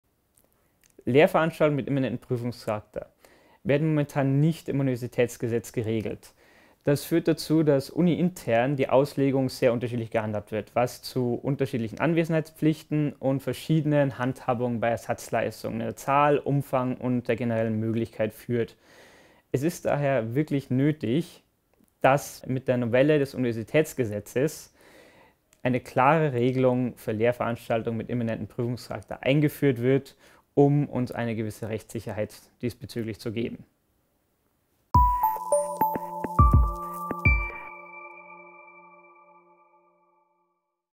LV mit immanentem Prüfungscharakter (Michael)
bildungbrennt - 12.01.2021

Lehrveranstaltungen mit ‘immanentem Prüfungscharakter’ sind ein Paradebeispiel für Prüfungen, die nicht als solche durch das Universitätsgesetz definiert sind. Während es für ‘Prüfungen’ eine gesetzliche Regelung gibt (Recht auf drei Termine pro Semester, Prüfungseinsicht bzw. Prüfungsprotokolleinsicht, usw.), ist der immanente Prüfungscharakter im Universitätsgesetz kaum geregelt. Als Folge dessen haben wir 100%ige Anwesenheitspflicht und ein uneinheitliches Ersatzleistungsprozedere, das zu unverhältnismäßigen Ersatzleistungen führt.

Eine gesetzliche Konkretisierung des immanenten Prüfungscharakters im Universitätsgesetz ist daher dringend notwendig.